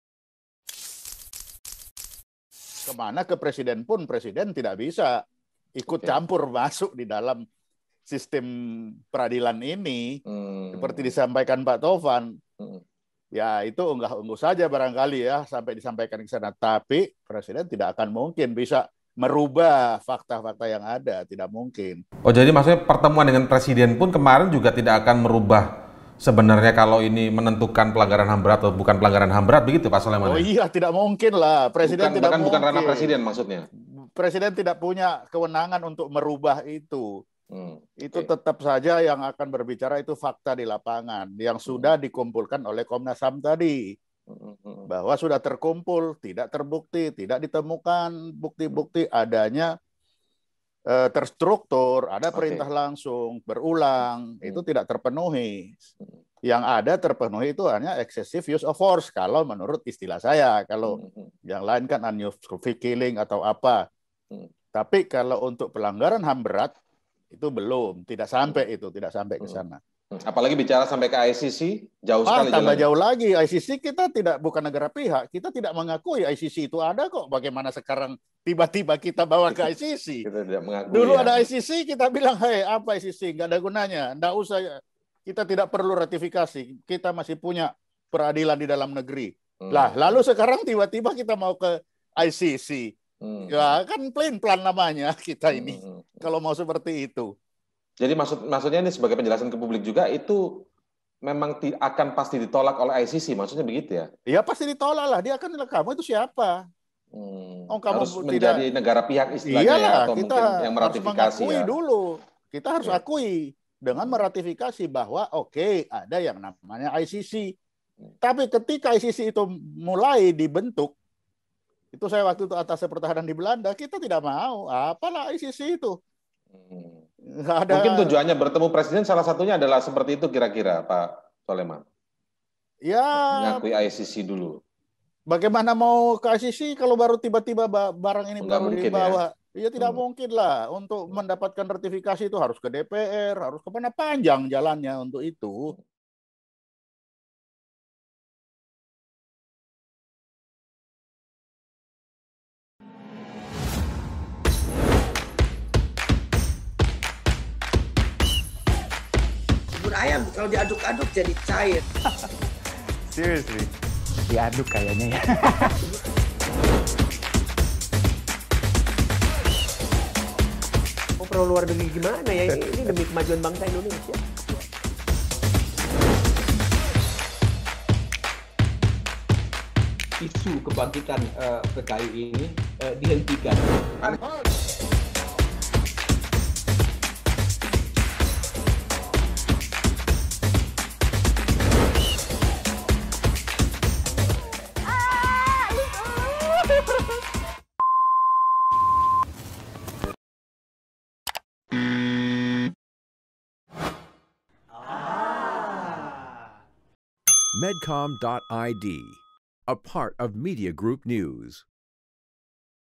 Presiden Tak Punya Wewenang Menetapkan Kasus Pelanggaran HAM Berat
Mantan Kabais TNI Soleman Ponto menjelaskan Presiden Joko Widodo tidak memiliki wewenang untuk menetapkan kasus pelanggaran HAM berat. Presiden tidak bisa ikut campur dalam sistem peradilan.

Sehingga pertemuan TP3 dengan Presiden Jokowi tidak bisa mengubah kasus penembakan enam laskar FPI menjadi pelanggaran HAM berat. Hanya hasil investigasi Komnas HAM yang bisa menentukan pelanggaran HAM tersebut.